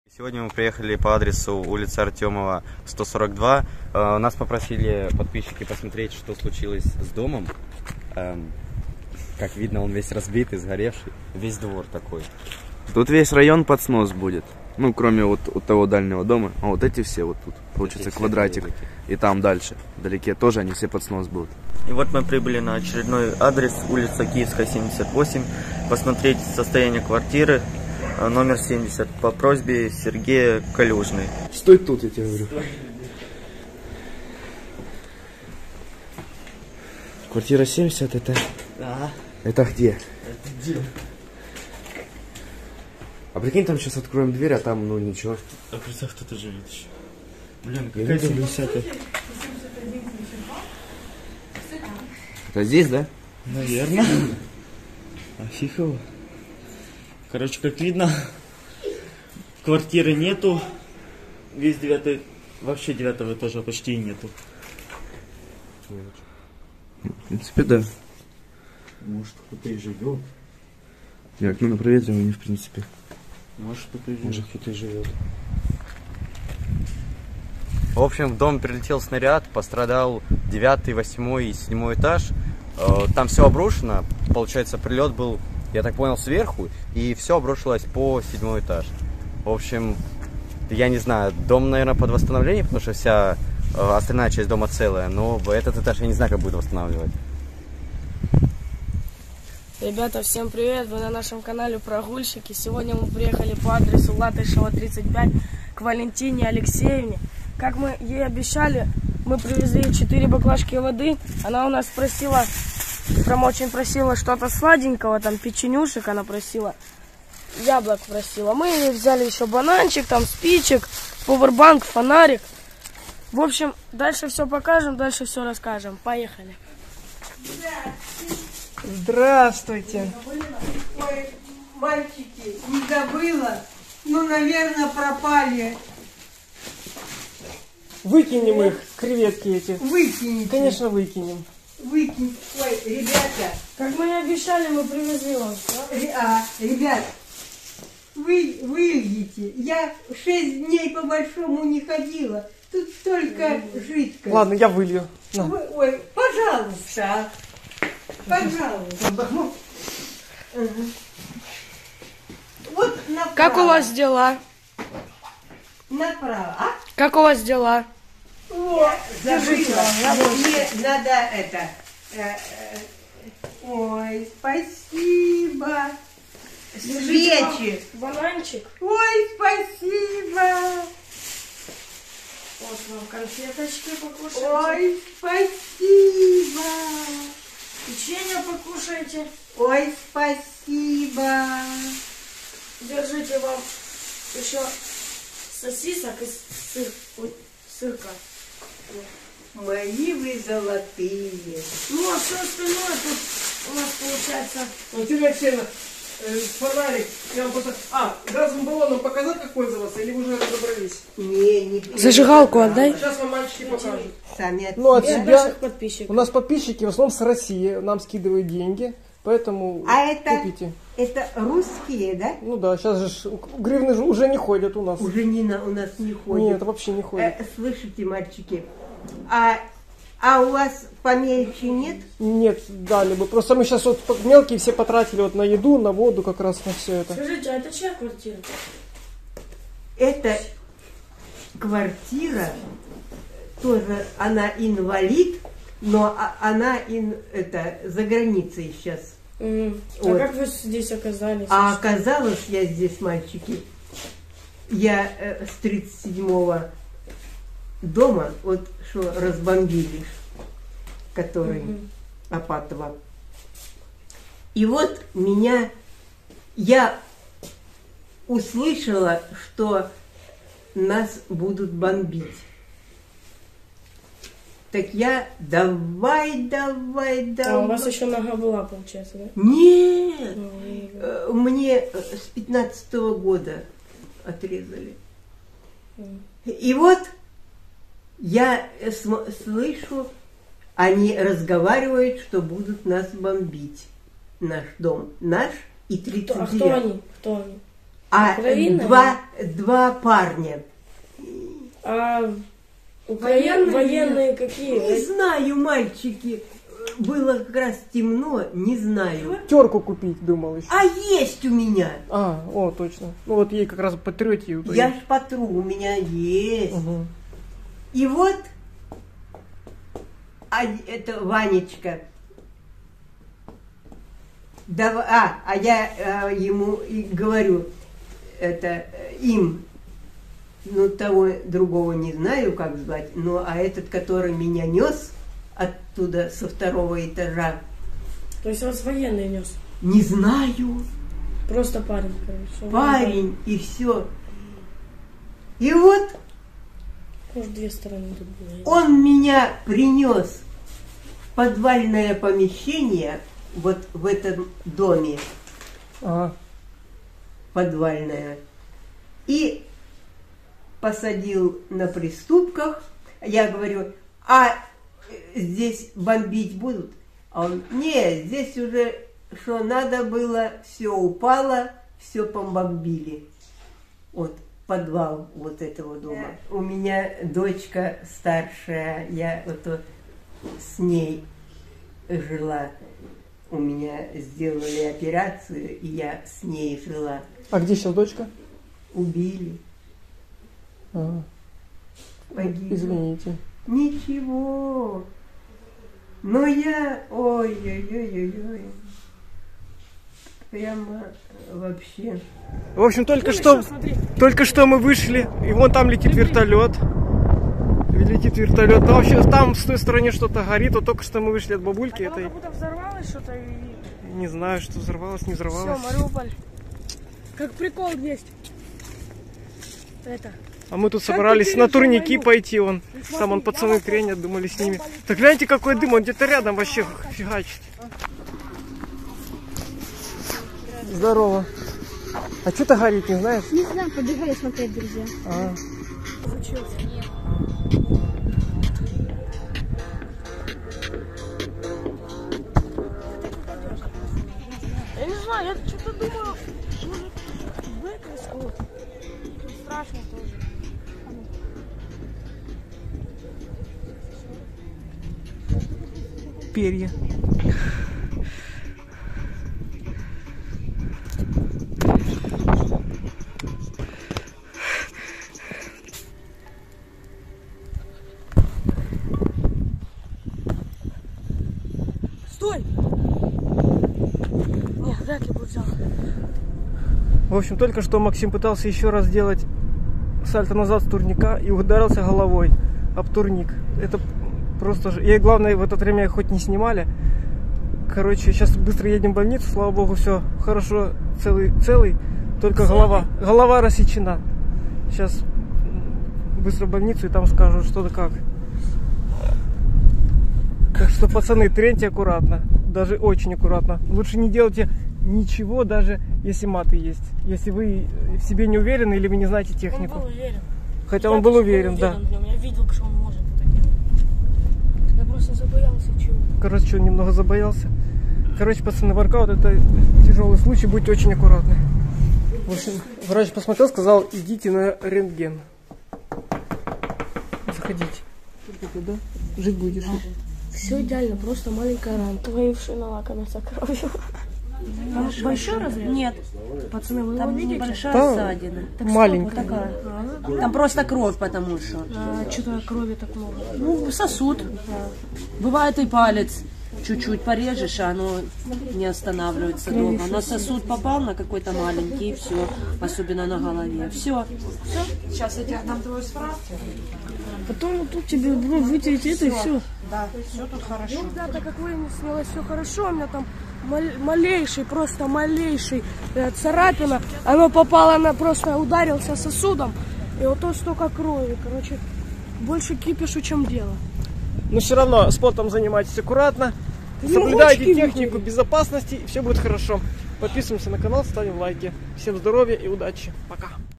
2 ноября прошлись по адресам, к Валентине Алексеевне и лечили Макса после падения
Сегодня трудный день, побывали на Киевской 78, Артёма 142, Латышева 35 у бабули и Макса везли в больницу после турников.
Ребят вы постепенно учитесь. Пусть кто то страхует во время обучения. У каждого свой организм и к каждому нужен свой подход. Берегите себя!
На конфеты любой помощи будем рады
карта Сбербанк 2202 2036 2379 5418 С. Александр Александрович.
ДЮК Спутник, г. Мариуполь
Мы в инстаграм: @progulshchiki